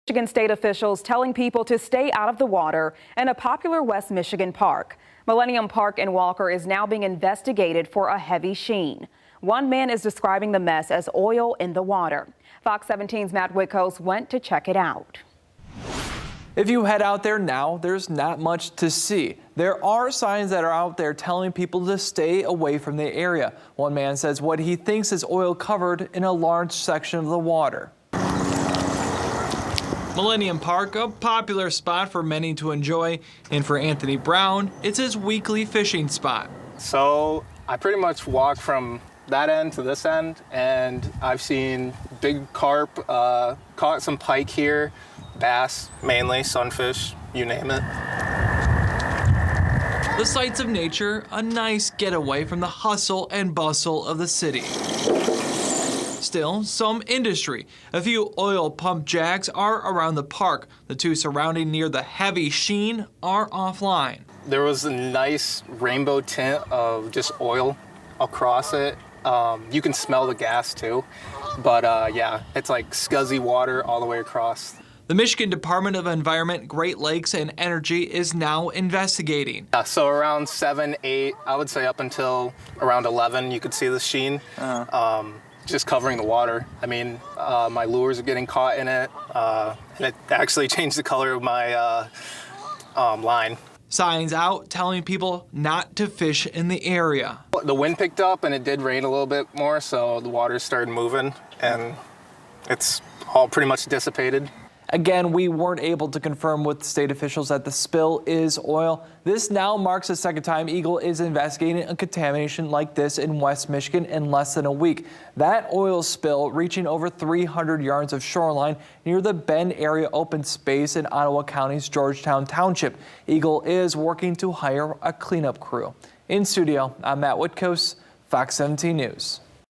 EGLE investigates sheen on pond in Millennium Park
State officials are warning community members and visitors to stay out of the water at a popular West Michigan park.